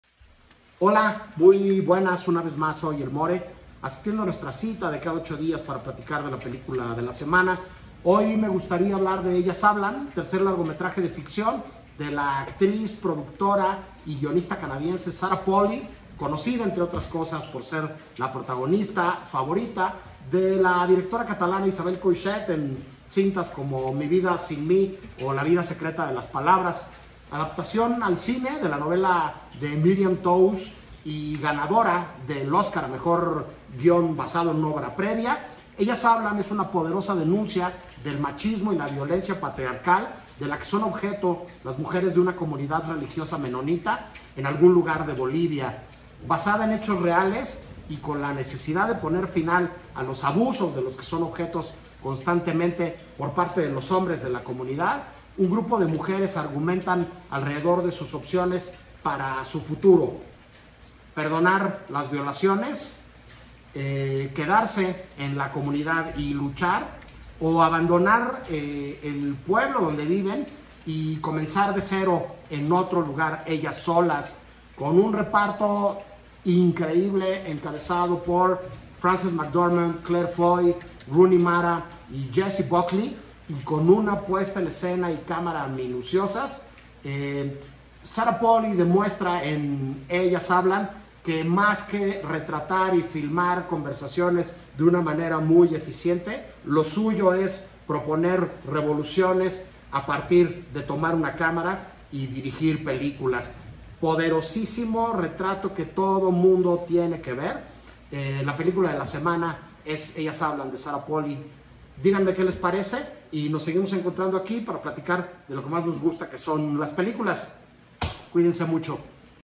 La película de la semana: Ellas hablan.
Poderosa y necesaria denuncia sobre el abuso y la violencia del machismo que usa una comunidad menonita como alegoría para hablar de la injusticia de toda la sociedad. Disponible en @CinetecaMexico @Cinepolis @Cinemex y @cinetonala